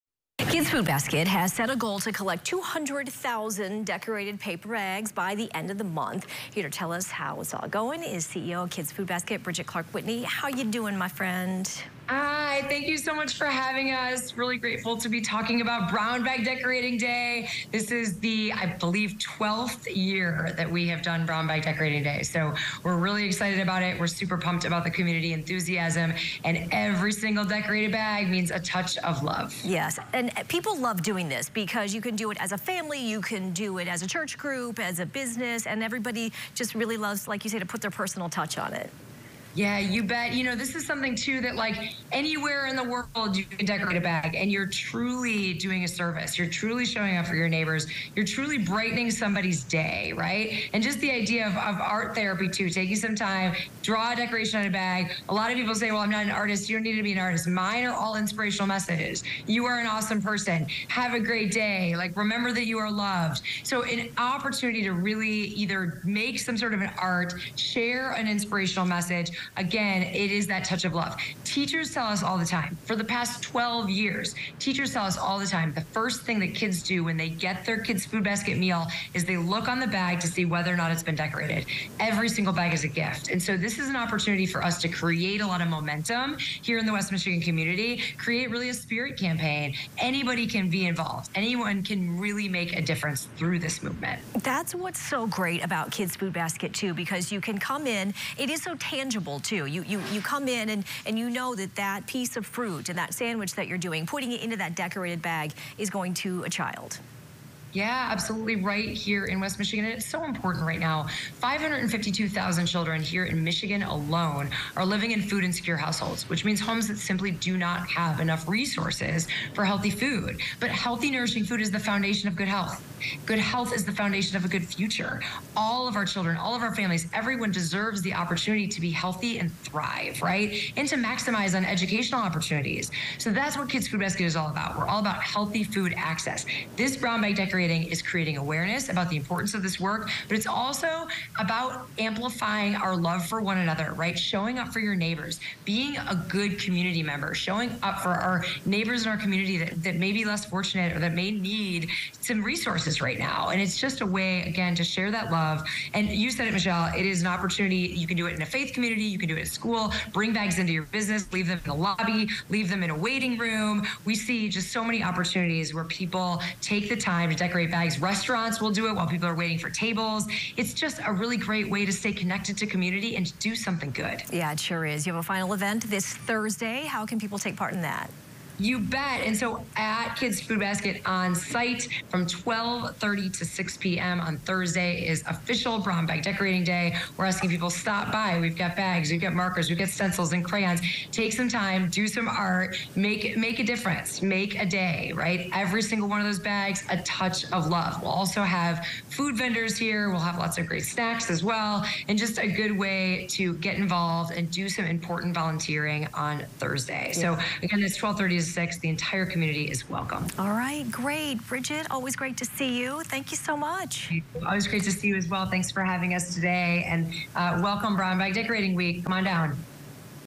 Kids' Food Basket aims to decorate 200K bags for kids
A Grand Rapids nonprofit that works to feed children is getting closer to its goal of decorating 200,000 paper bags this month — but it still needs your help. (Sept. 25, 2023)